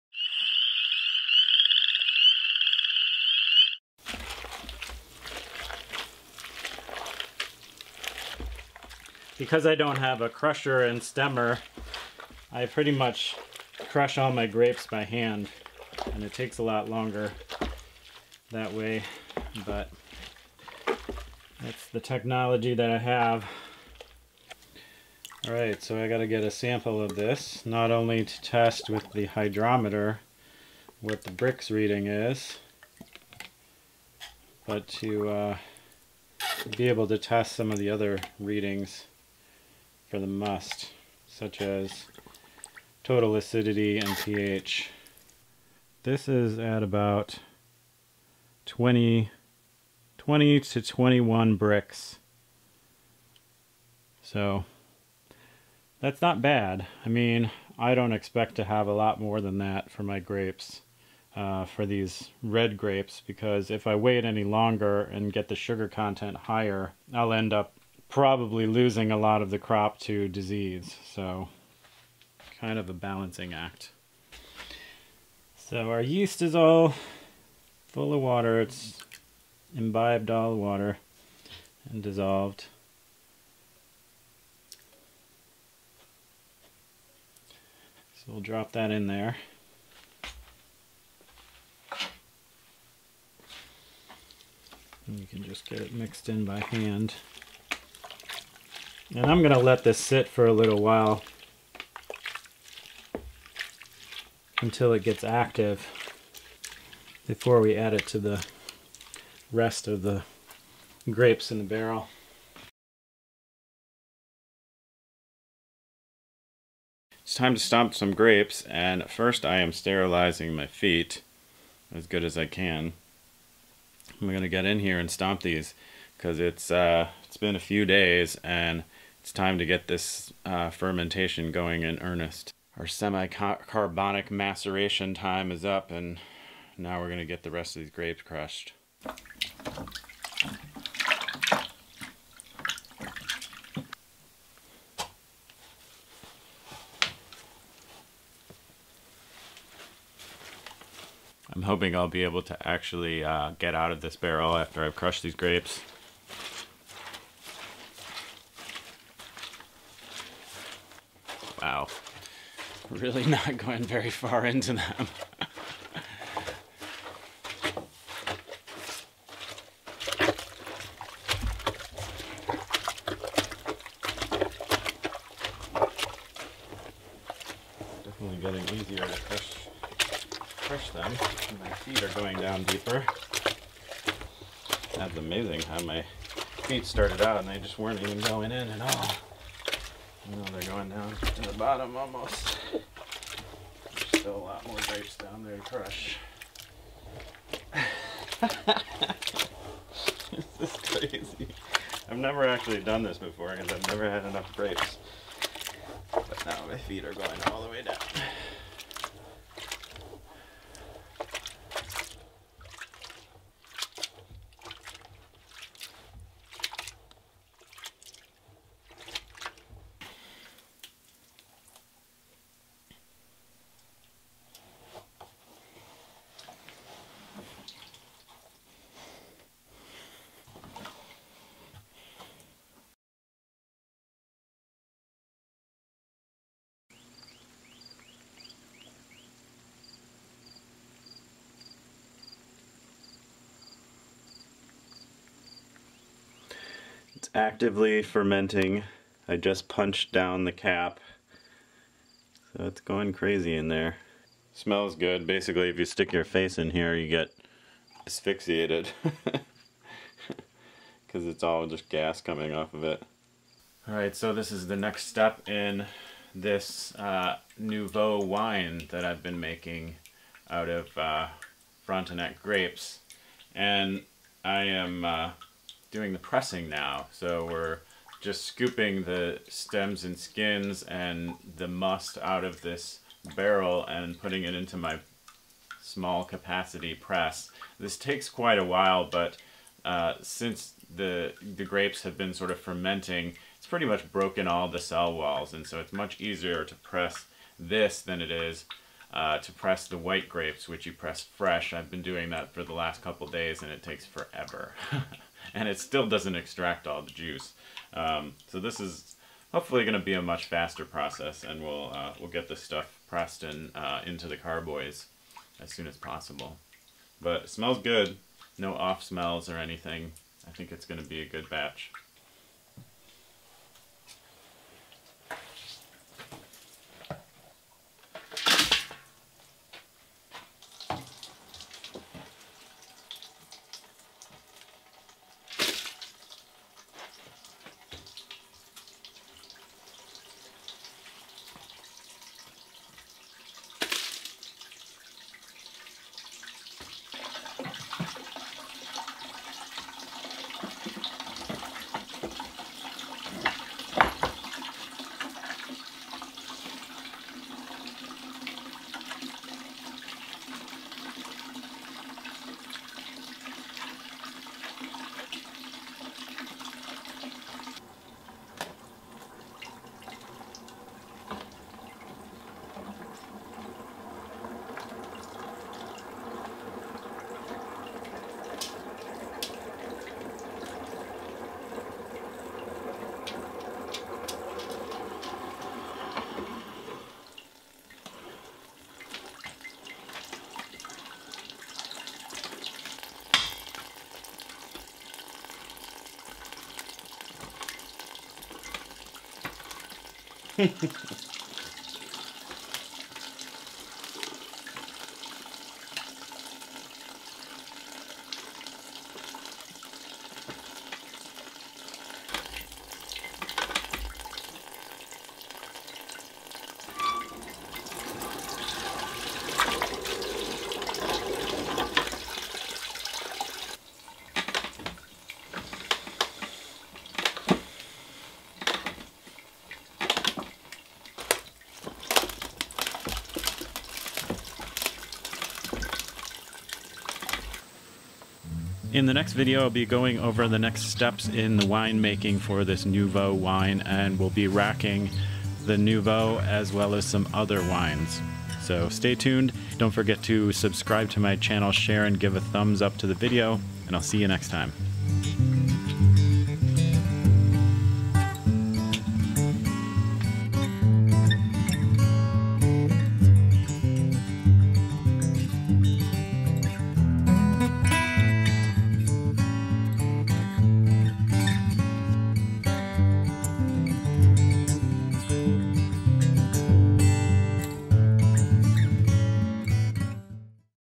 Stomping Grapes for Organic Nouveau Wine Part 2
Continuing the process of making nouveau wine out of my homegrown organic grapes, in this video I stomp, ferment, and press the grapes.  Though the thumbnail may look like I'm trying to pinch a loaf, I'm actually wondering if I'll be able to get out of the barrel I'm stomping grapes in.  In case you are wondering--spoiler alert--I made it out.